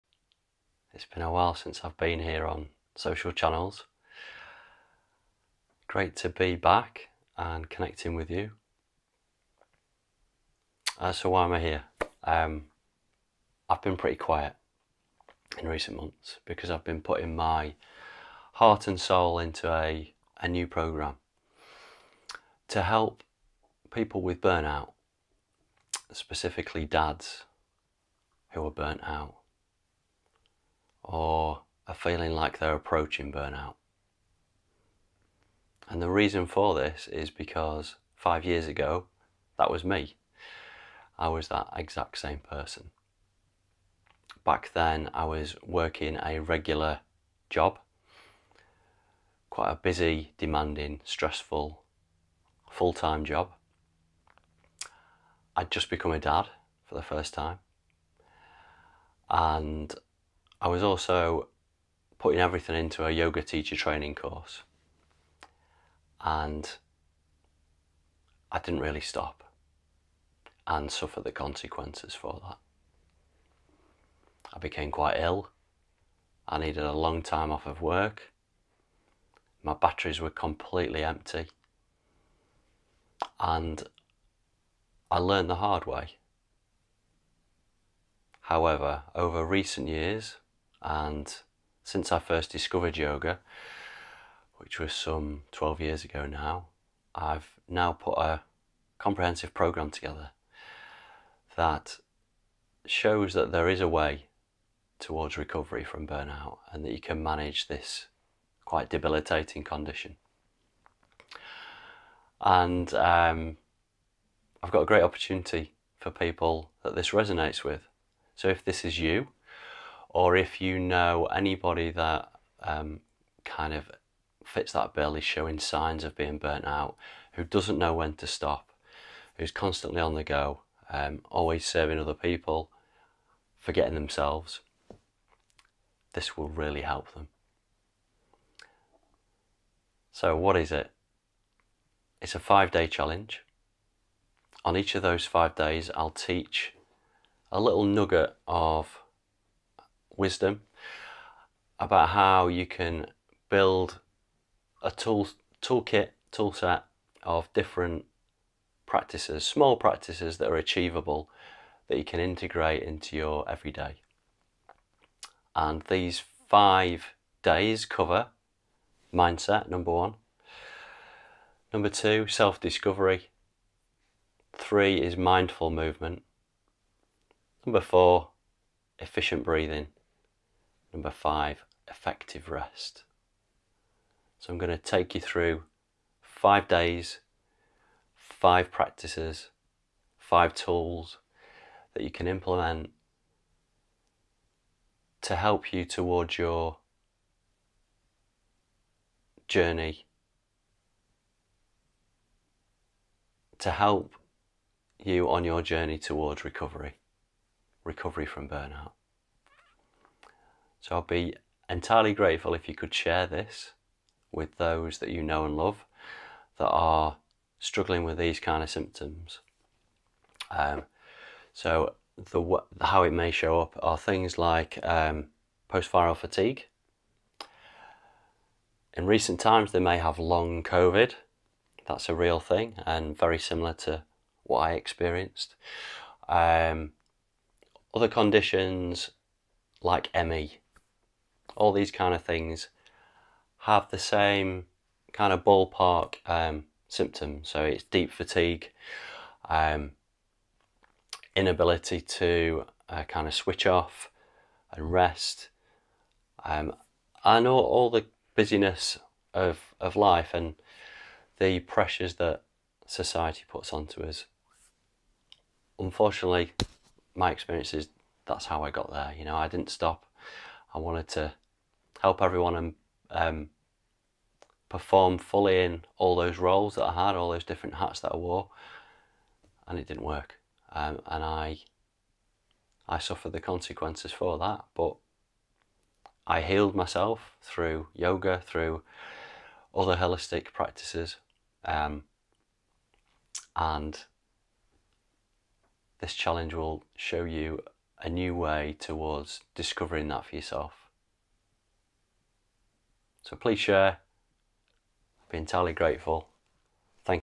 INTRO - The Burn Out Recovery Challenge
The Burn Out Recovery Challenge - Introduction

For further wisdom sharing on burn out, I lovingly invite you to join my Facebook group

If you'd like to delve deeper into how you can achieve recovery from burn out, click here to register for my 40 minute masterclass